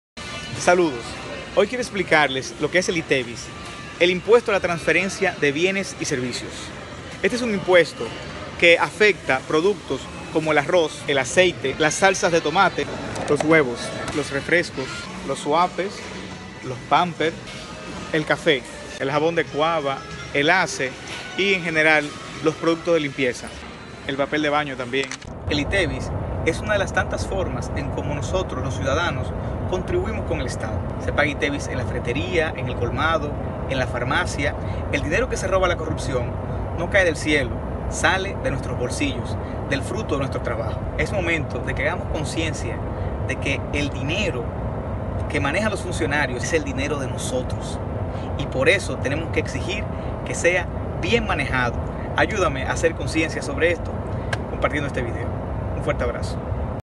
¿Sabes que el ITBIS?
Claudio Caamaño Vélez explica que es el impuesto a la transferencia de bienes y servicios